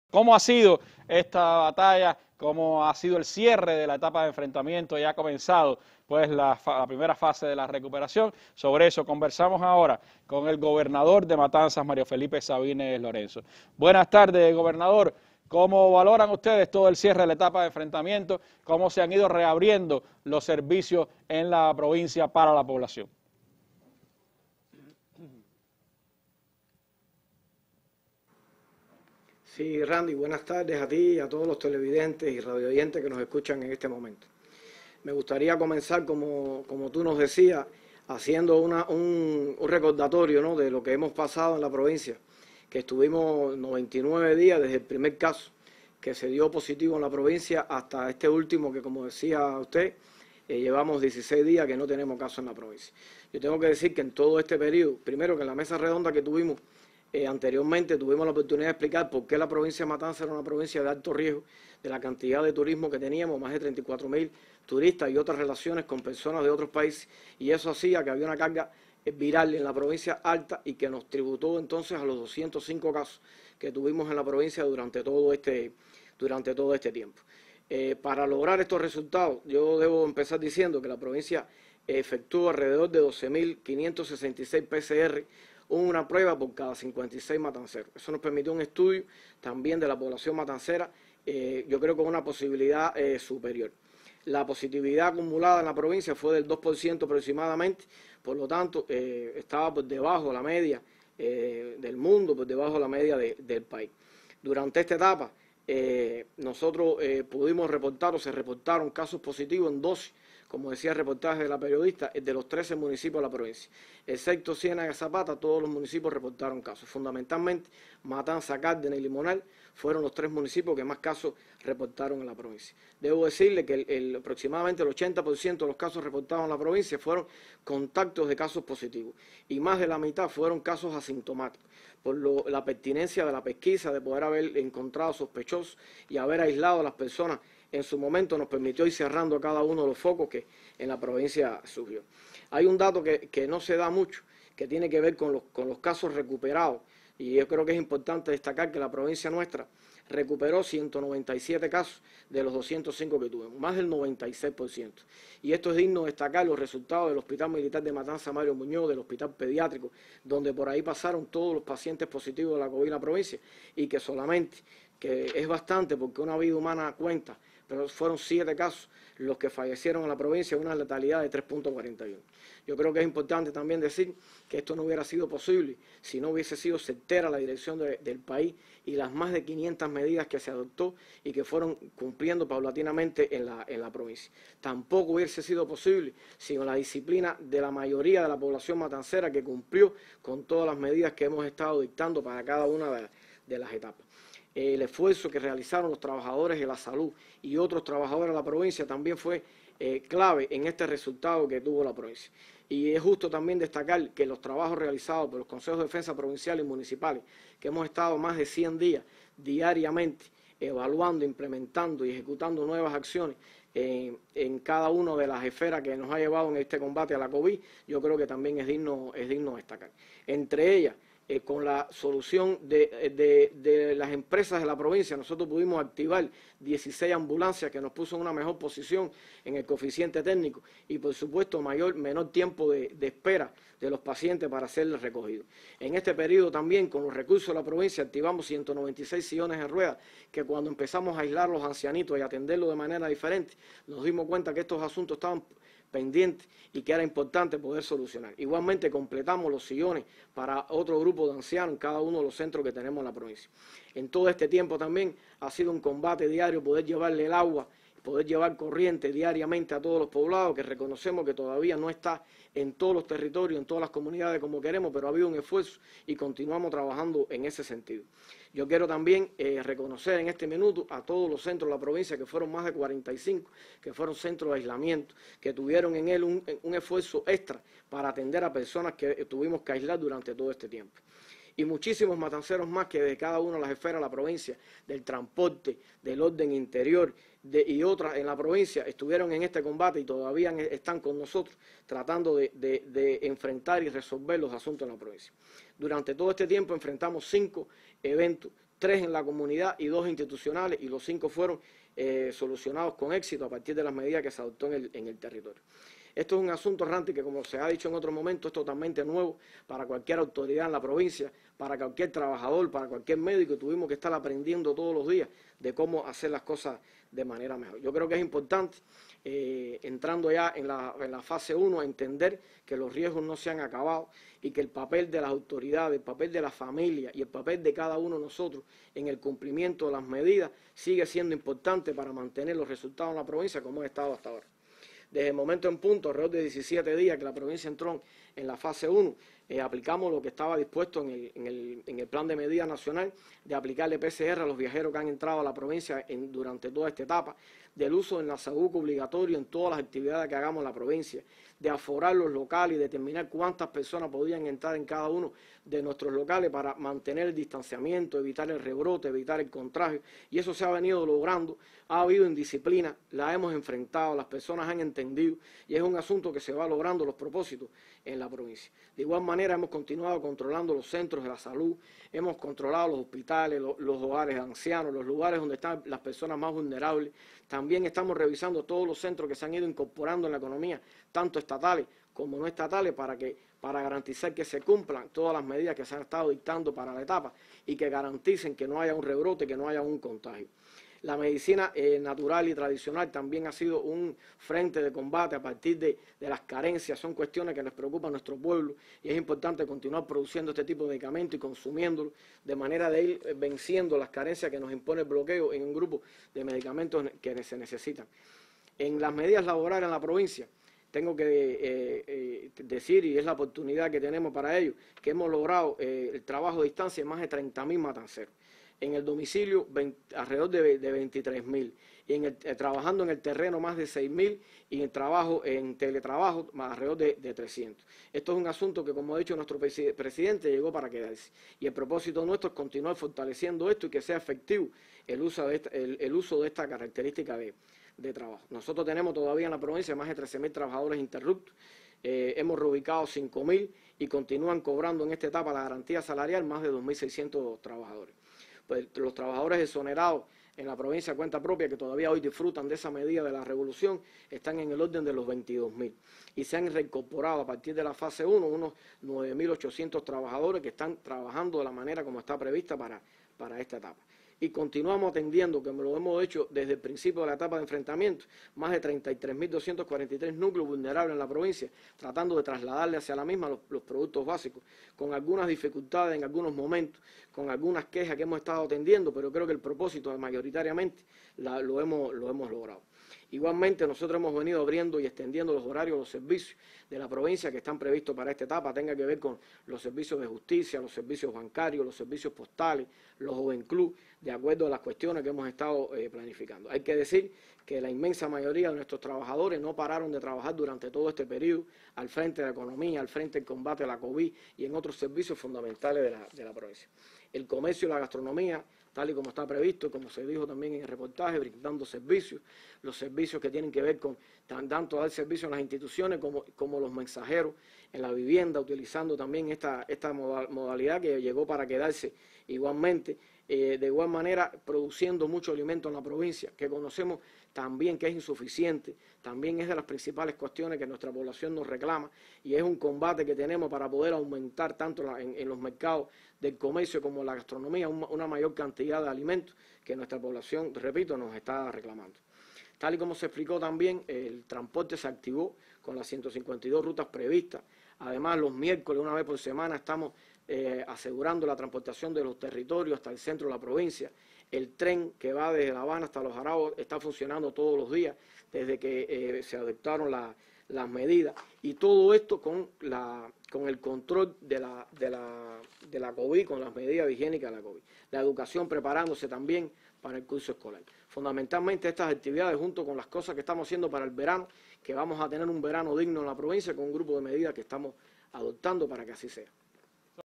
¿Cómo se enfrenta en Matanzas la COVID19?
El Gobernador de Matanzas, Mario Felipe Sabines Lorenzo, ofrece detalles del cierre de la etapa de enfrentamiento a la COVID-19 en ese territorio, así como de la primera fase de recuperación.